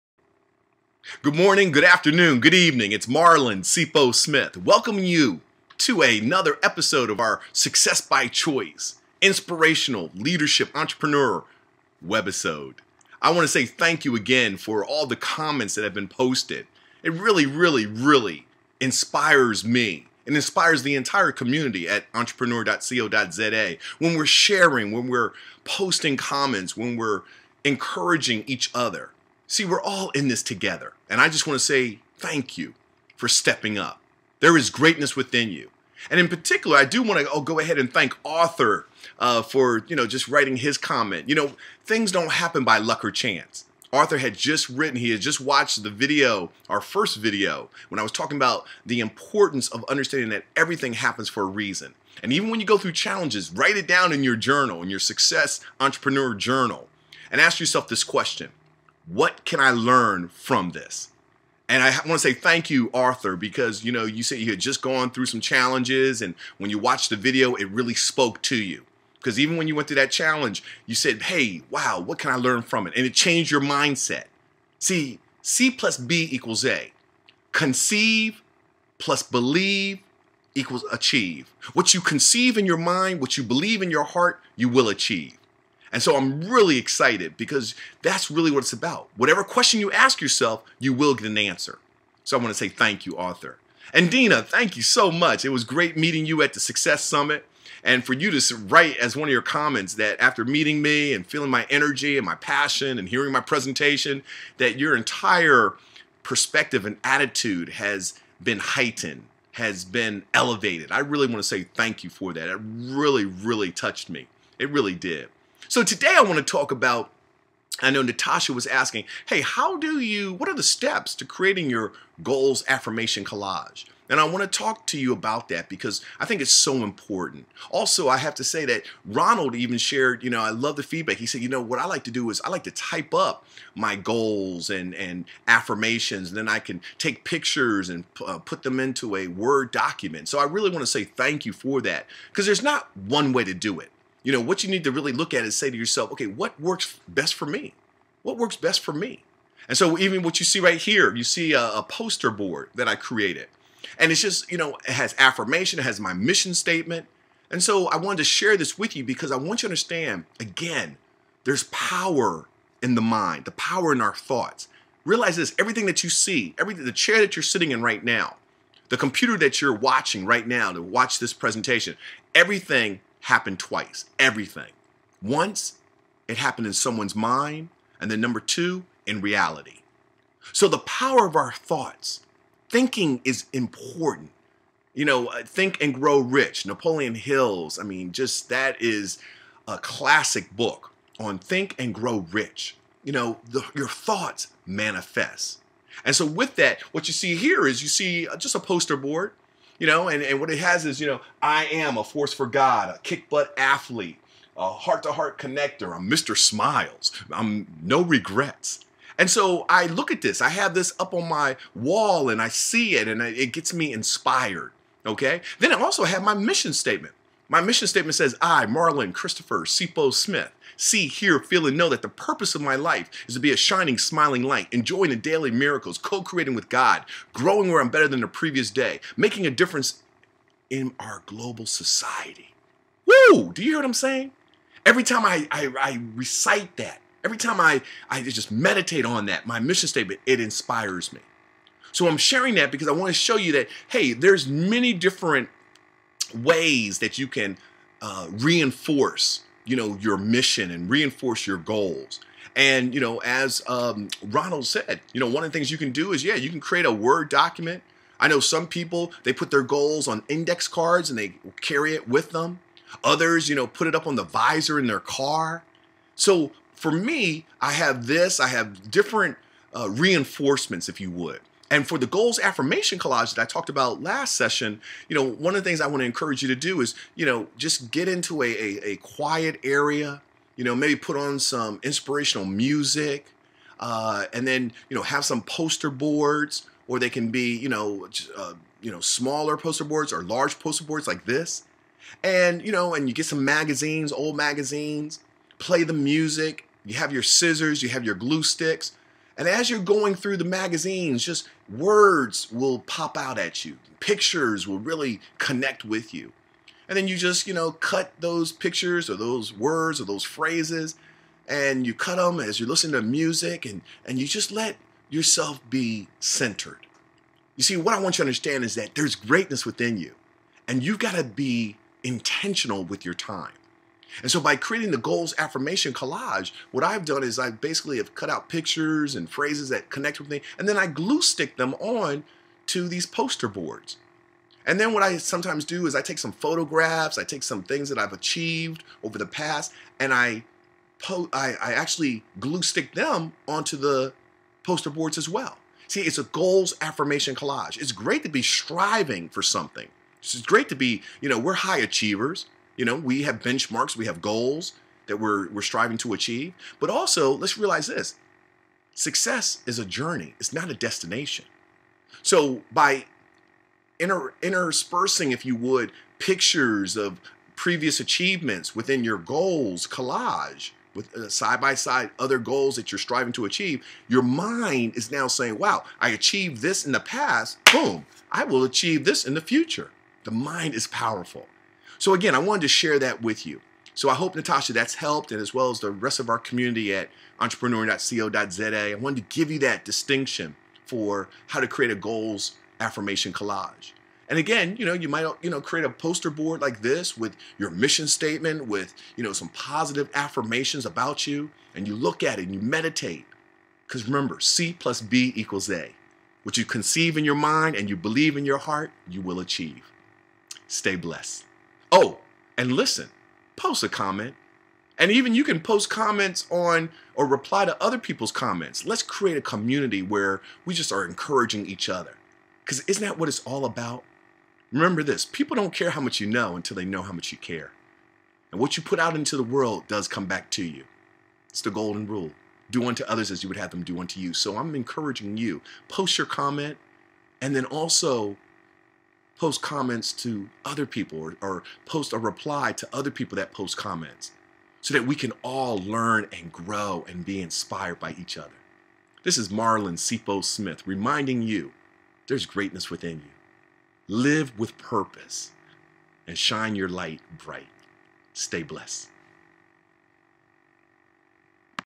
Success By Choice webinar episode #3
In this episode, Marlon reinforces the importance of maintaining a winning mindset.  To help individuals maximize their potential, Marlon shares steps for creating your GOALS - AFFIRMATION COLLAGE.  Feel free to post your comments.